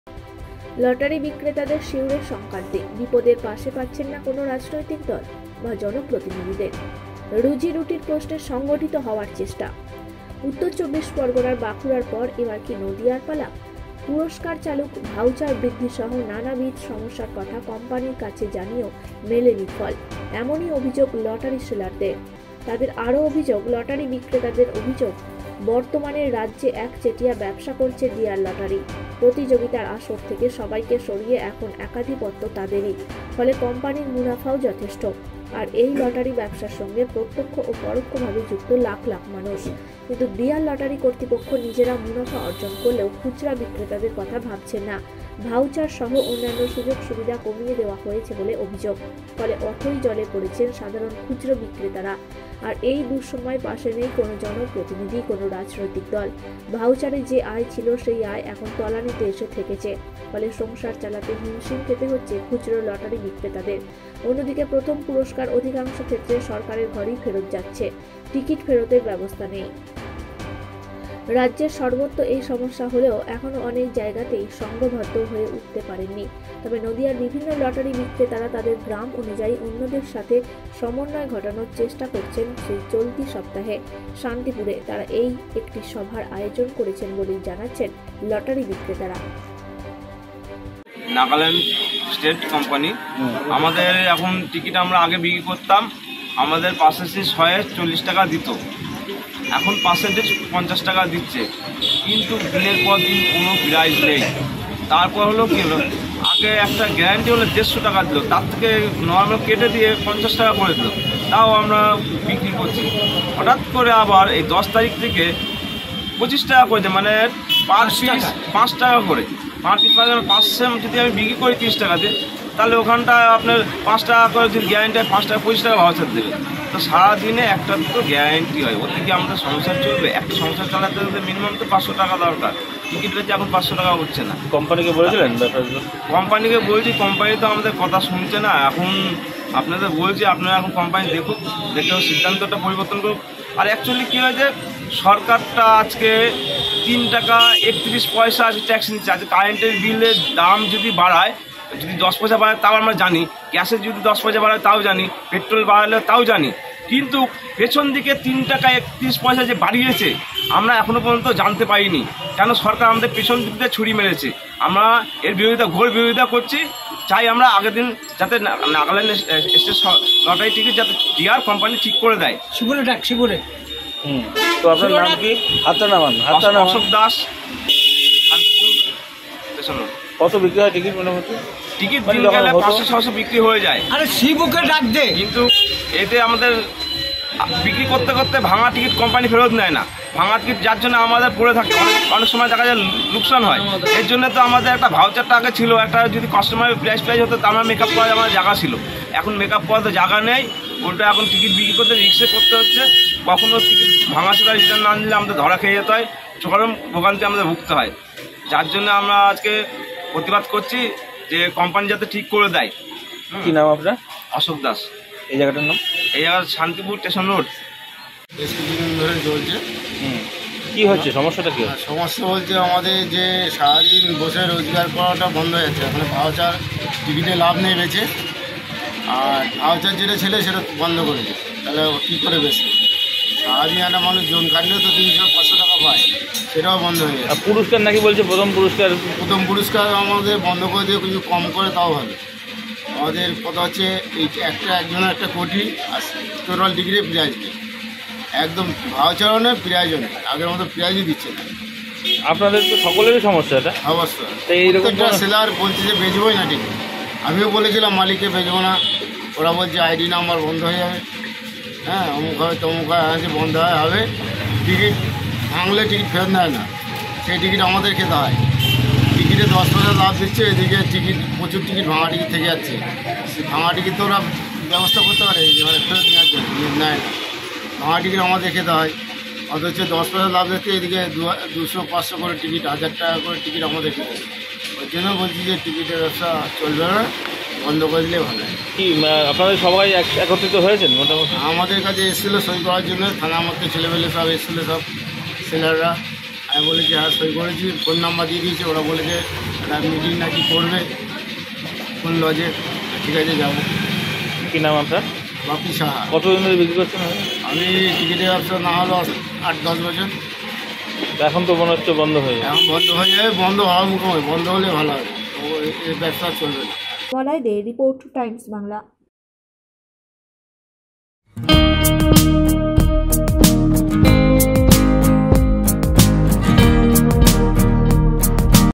লটারি বিক্রেতাদের শিহরে সংক্রান্তি, বিপদে পাশে পাচ্ছেন না কোন রাজনৈতিক দল বা জনপ্রতিনিধিদের
লটারি বিক্রেতাদের শিহরে সংক্রান্তি, বিপদে পাশে পাচ্ছেন না কোন রাজনৈতিক দল বা জনপ্রতিনিধিদের, রুজি রুটির প্রশ্নে সংগঠিত হওয়ার চেষ্টা